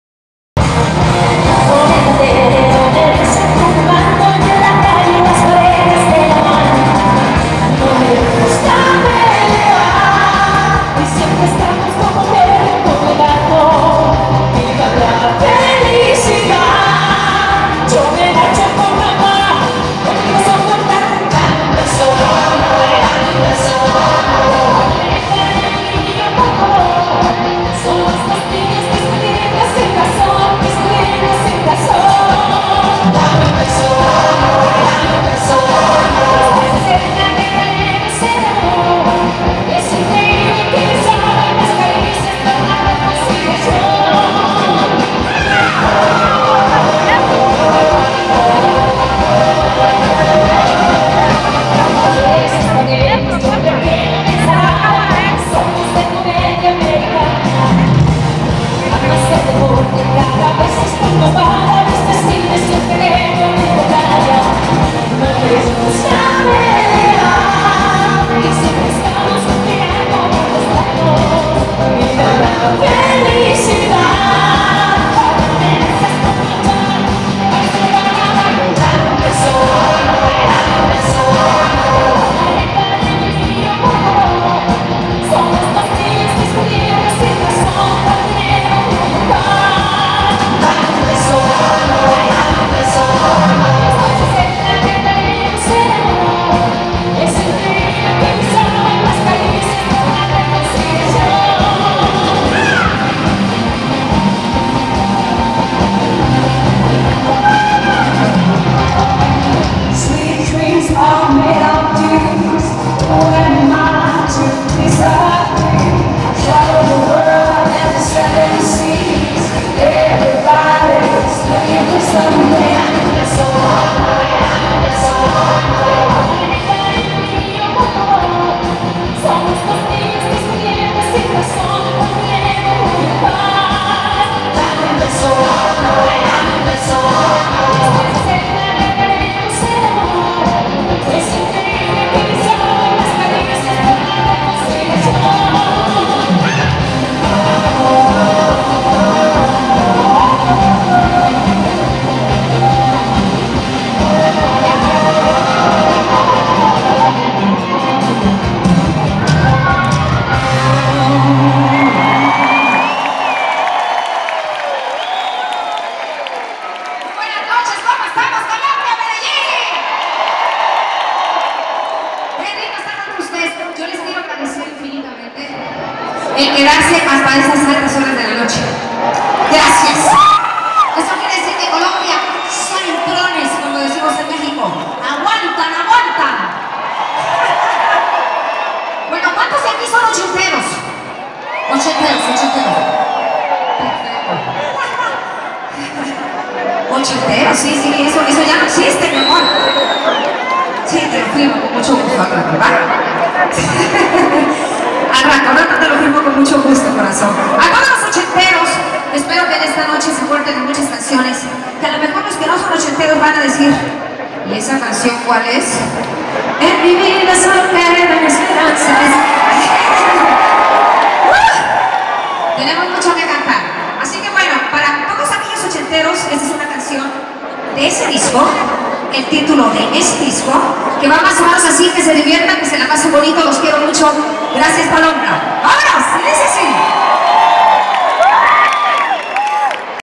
YURI - 12 Aniversario de Melodía para Dos - Medellín Octubre 3, 2015 La Tiquetera
Concierto Melodía Para Dos Octubre 3, 2015 La Tiquetera - Vídeo Bar - El mejor Vídeo Bar de Antioquia.
Pioneros en música romántica de los años 60s y 70s.
Presenta su Concierto 12 Aniversario de Melodía para ​Dos
Myriam Hernández - Cristian Castro - Yuri y Chris Vahos.